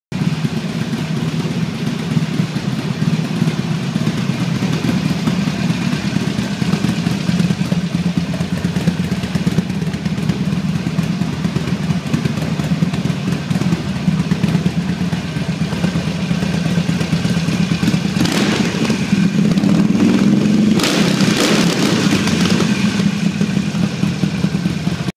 2007 XL1200C de baffled stock exhaust
My 2007 sportster custom with baffles completely removed.